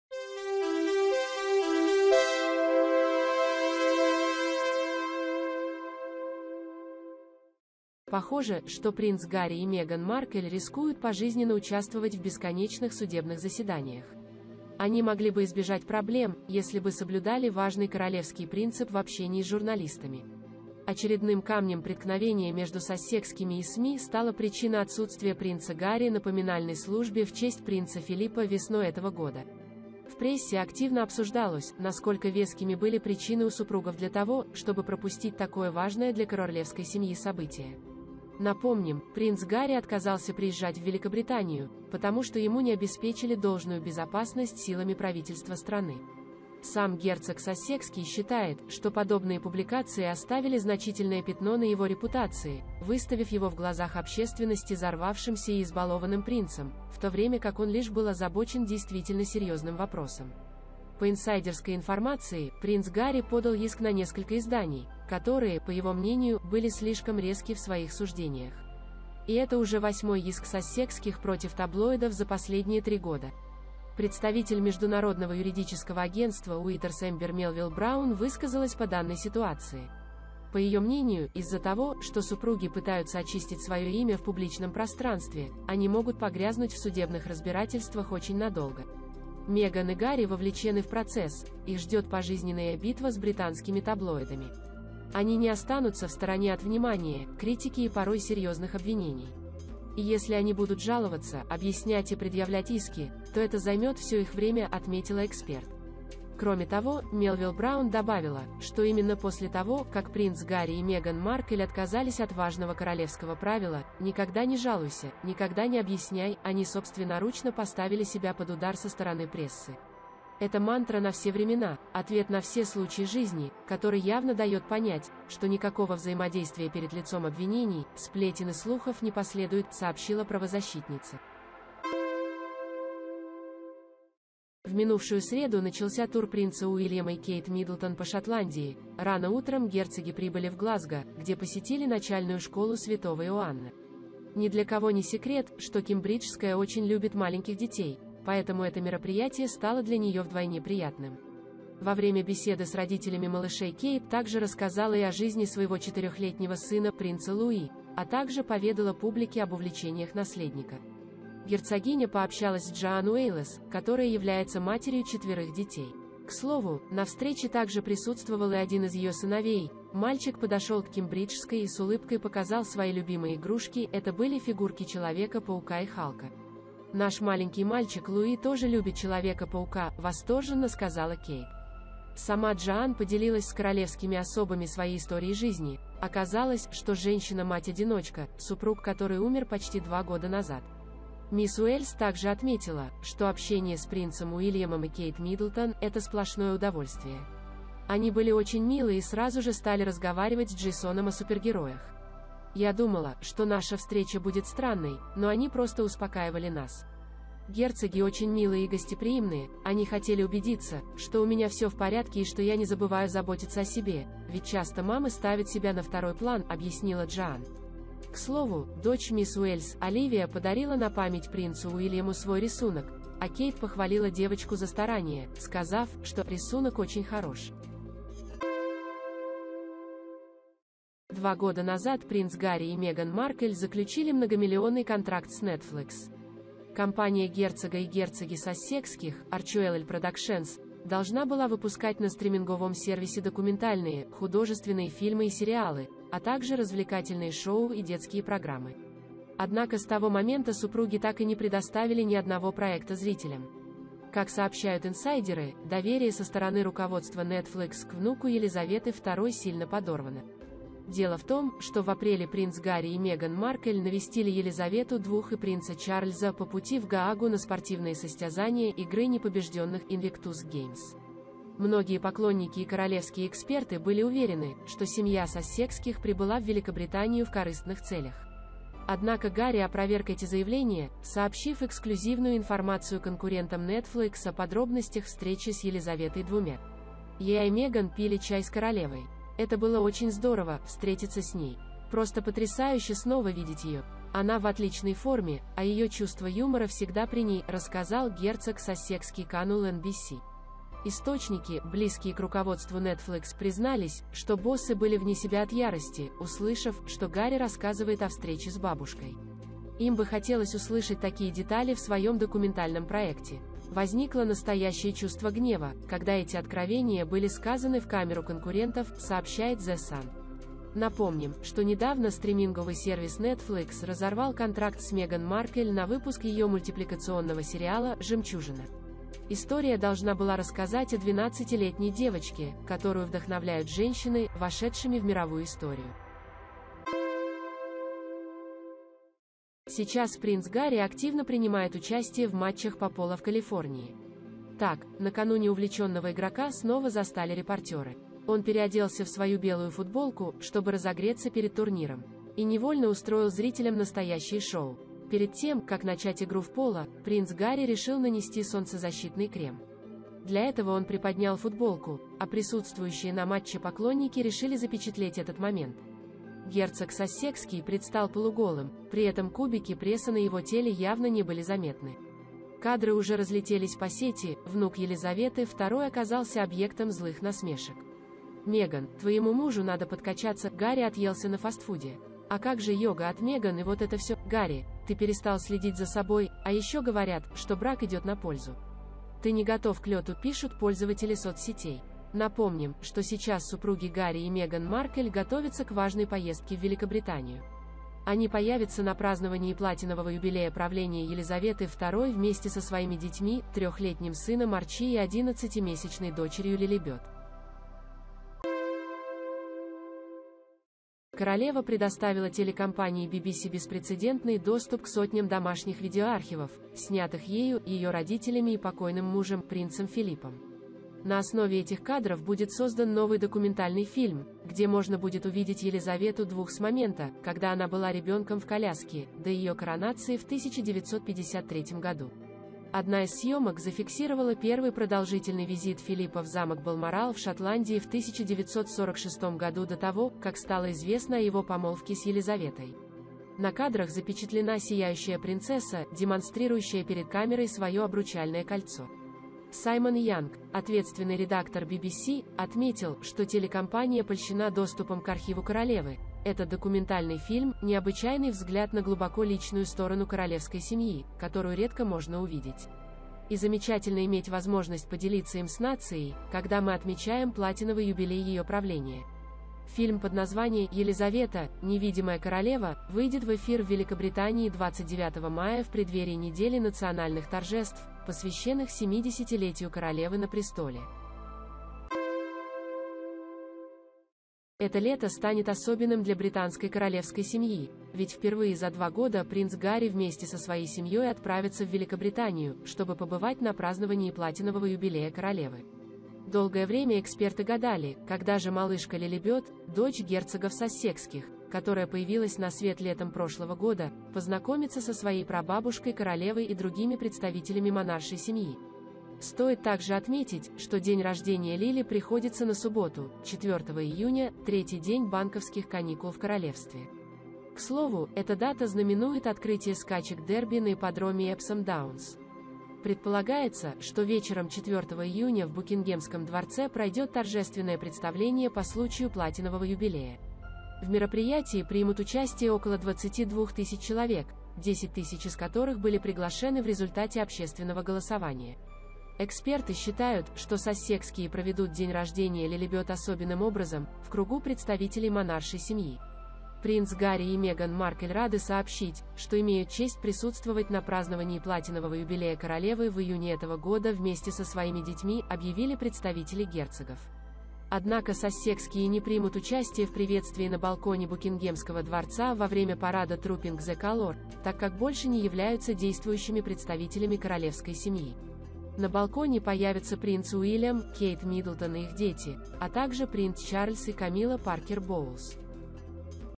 Принц Гарри и Меган Маркл вновь воюют с британскими таблоидами. Что на этот раз
Похоже, что принц Гарри и Меган Маркл рискуют пожизненно участвовать в бесконечных судебных заседаниях. Они могли бы избежать проблем, если бы соблюдали важный королевский принцип в общении с журналистами.
Очередным камнем преткновения между Сассекскими и СМИ стала причина отсутствия принца Гарри на поминальной службе в честь принца Филиппа весной этого года. В прессе активно обсуждалось, насколько вескими были причины у супругов для того, чтобы пропустить такое важное для корорлевской семьи событие. Напомним, принц Гарри отказался приезжать в Великобританию, потому что ему не обеспечили должную безопасность силами правительства страны.
Сам герцог Сассекский считает, что подобные публикации оставили значительное пятно на его репутации, выставив его в глазах общественности зарвавшимся и избалованным принцем, в то время как он лишь был озабочен действительно серьезным вопросом. По инсайдерской информации, принц Гарри подал иск на несколько изданий, которые, по его мнению, были слишком резки в своих суждениях. И это уже восьмой иск Сассекских против таблоидов за последние три года.
Представитель международного юридического агентства Withers Эмбер Мелвилл-Браун высказалась по данной ситуации. По ее мнению, из-за того, что супруги пытаются очистить свое имя в публичном пространстве, они могут погрязнуть в судебных разбирательствах очень надолго. «Меган и Гарри вовлечены в процесс, их ждет пожизненная битва с британскими таблоидами. Они не останутся в стороне от внимания, критики и порой серьезных обвинений. И если они будут жаловаться, объяснять и предъявлять иски, то это займет всё их время», — отметила эксперт.
Кроме того, Мелвилл-Браун добавила, что именно после того, как принц Гарри и Меган Маркл отказались от важного королевского правила «никогда не жалуйся, никогда не объясняй», они собственноручно поставили себя под удар со стороны прессы. «Это мантра на все времена, ответ на все случаи жизни, который явно дает понять, что никакого взаимодействия перед лицом обвинений, сплетен и слухов не последует», — сообщила правозащитница.